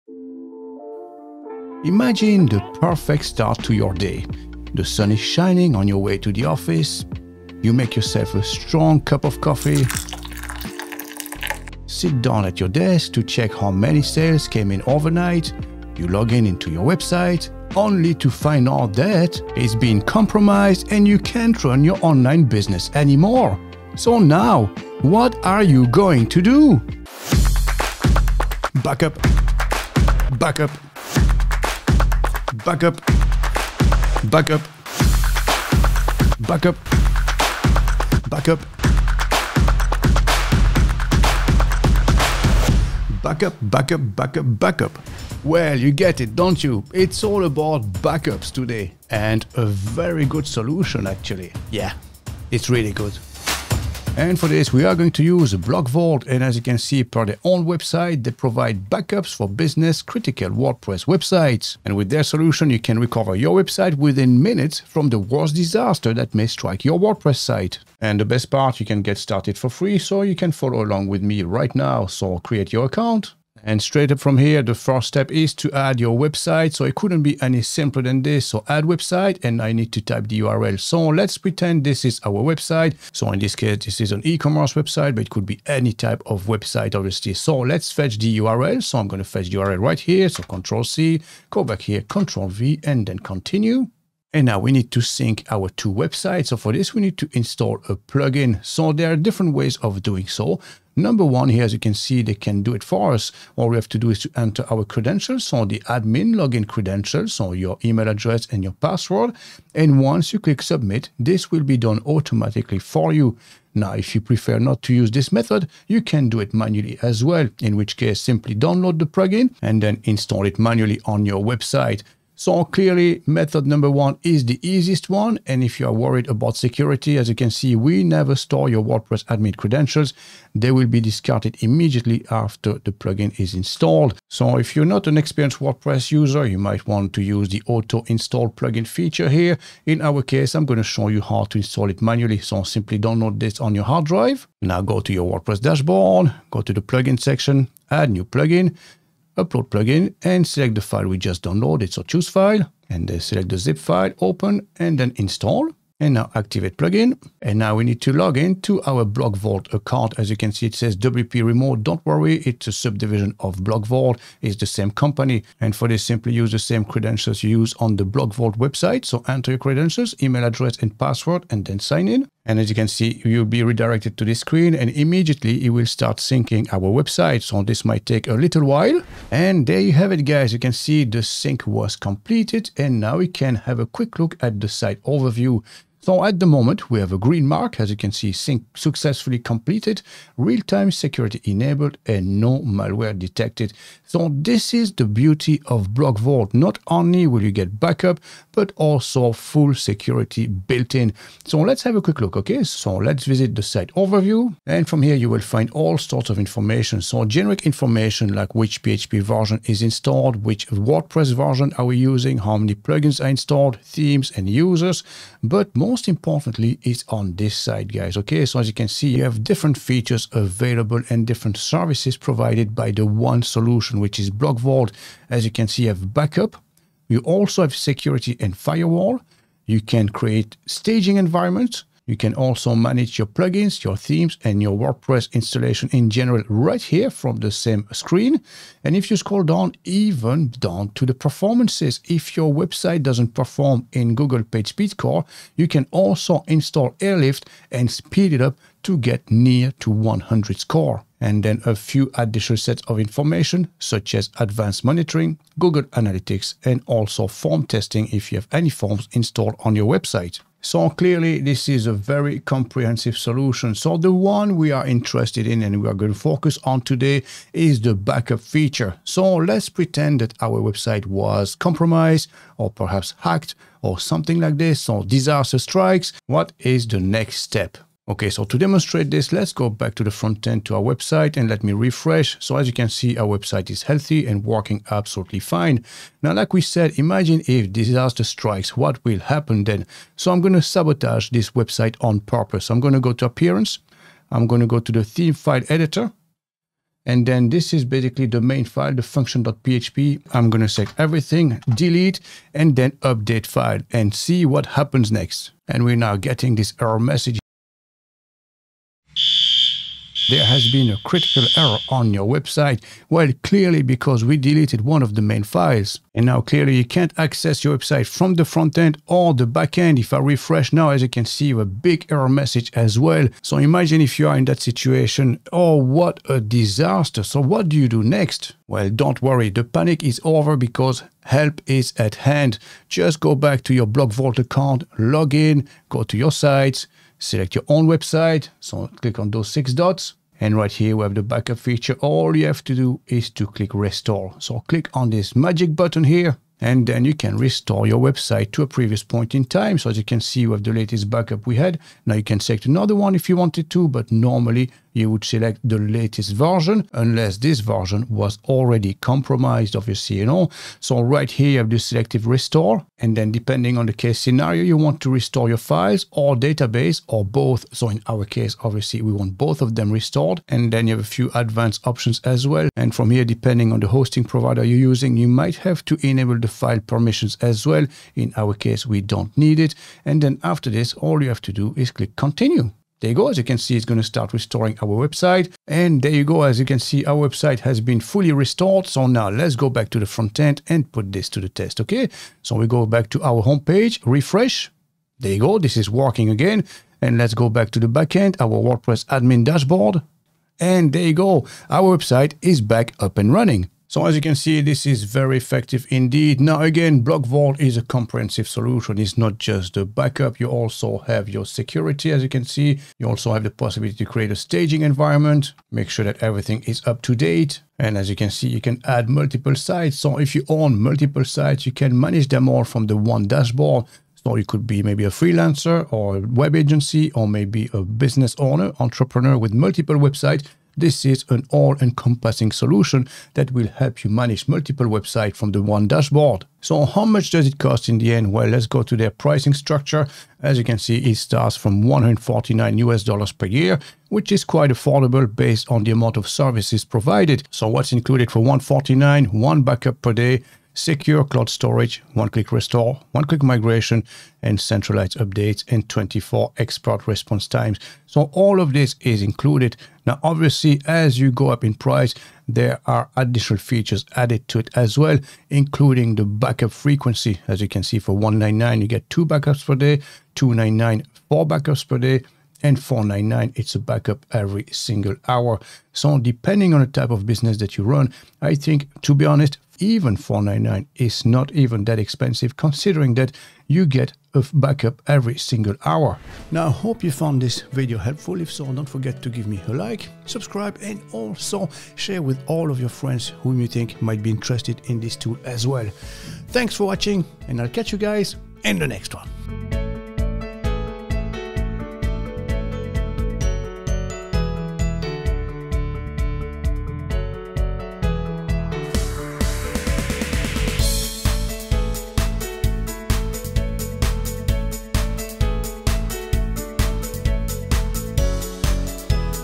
How to Backup a WordPress Website in 2025
Unlock the secrets to securing your WordPress website in 2025! In this video, we'll dive into the essential secrets of WordPress backups, featuring the powerful BlogVault plugin. Learn how to set up and configure BlogVault to ensure your website's safety and integrity. Discover the best practices for backing up and restoring your WordPress site, and get expert tips on how to secure your online presence. Whether you're a seasoned developer or a beginner, this video will walk you through the step-by-step process of using BlogVault to protect your WordPress blog. From setup to restoration, we'll cover it all. So, what are you waiting for? Watch now and take the first step towards a more secure online future!

⚡ Protect Your WordPress Site NOW with BlogVault!

💬 Have you ever lost a website due to a crash or hack?